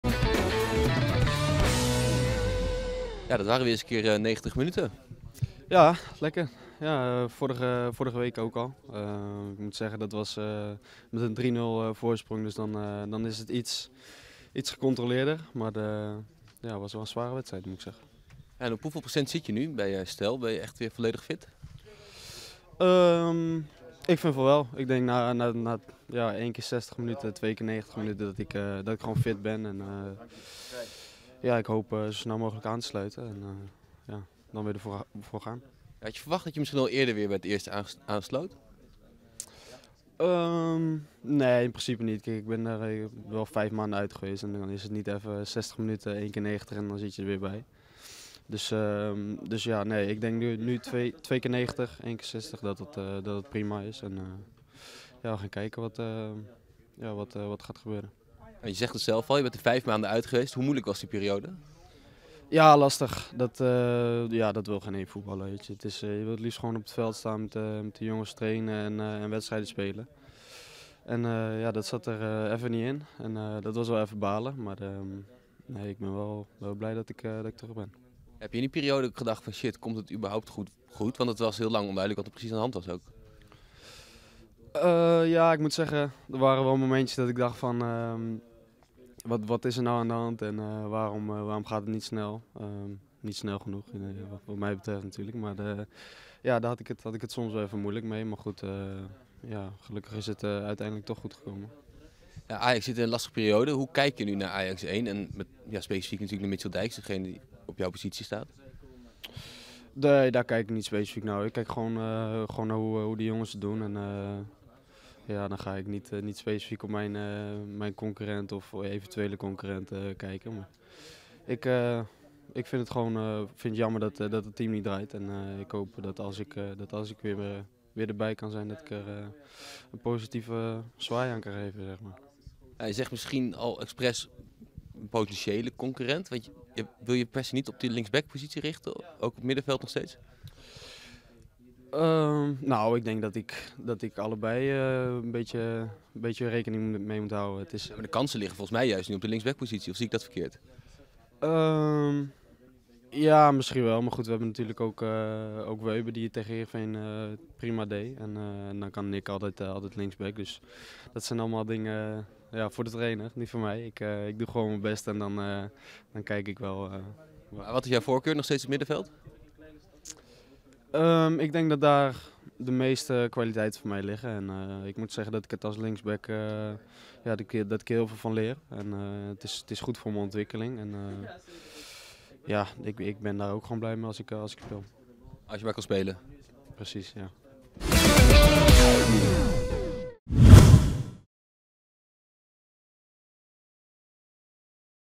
Daley Sinkgraven: ''Mijn kwaliteiten liggen op het - VOETBAL INSIDE
Clubinsider Daniel Schoonenboom sprak Daley Sinkgraven na afloop van de wedstrijd Jong Ajax - Jong FC Utrecht. De linkspoot lijk klaar voor een rentree in het eerste elftal.